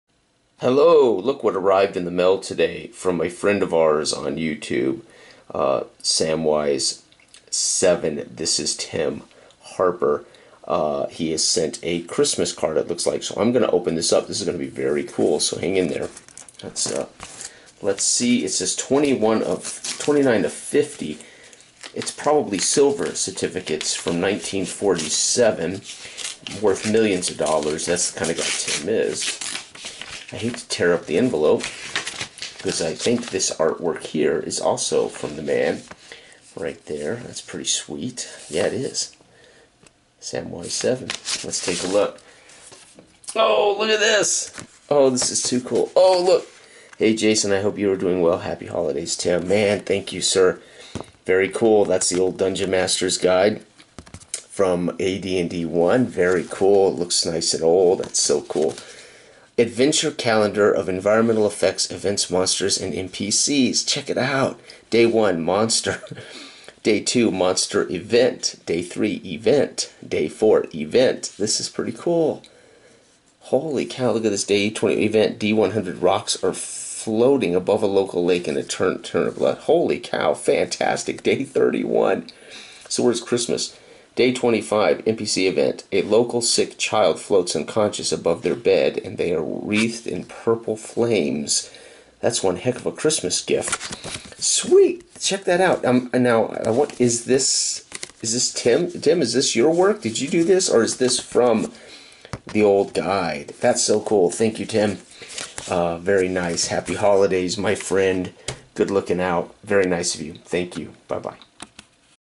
Thank You Tim! Happy Holidays!
Tim sends a very cool and unique holiday card.